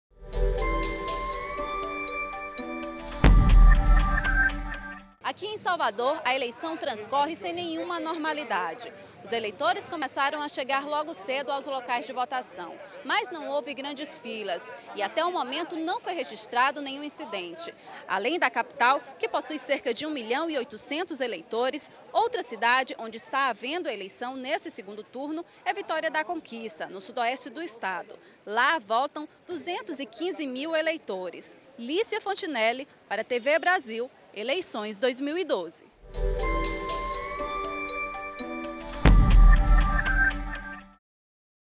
Salvador tem votação tranquila
Salvador possui mais de um milhão e 800 eleitores. A outra cidade da Bahia em que acontece segundo turno é Vitória da Conquista. Mais informações com Lícia Fontinelli, diretamente da capital Baiana.

Acompanhe o resultado das Eleições 2012 em tempo real pelo Portal EBC